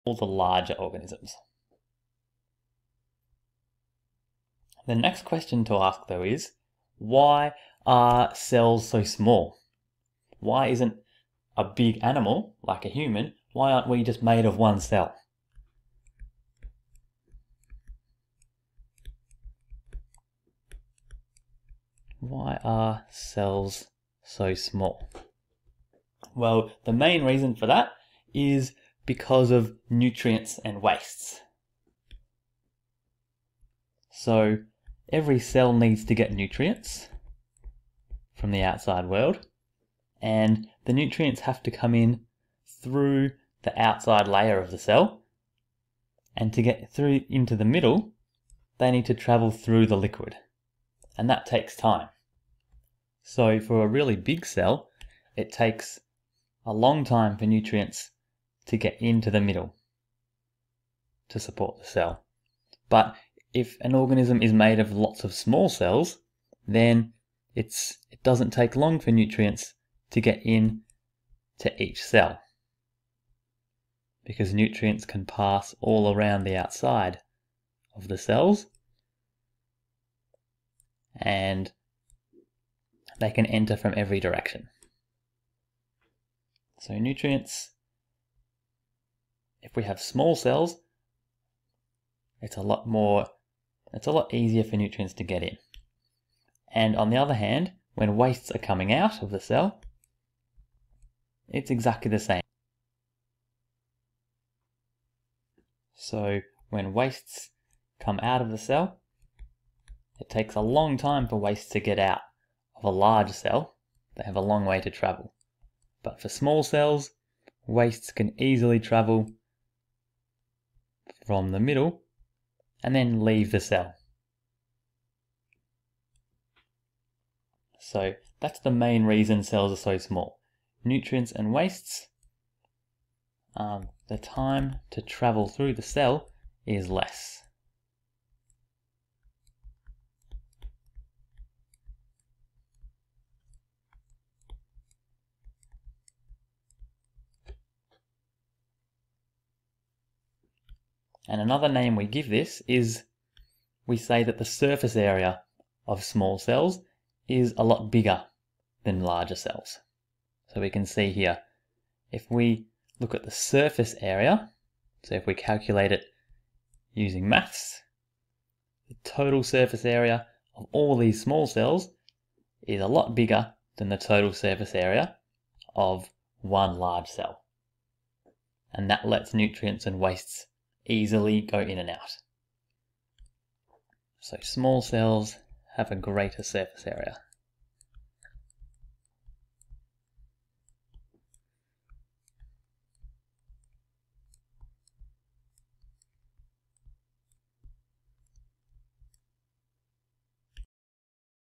Why Cells are so Small
Learning Intention Question:
Q1 Explain the main reason why cells are so small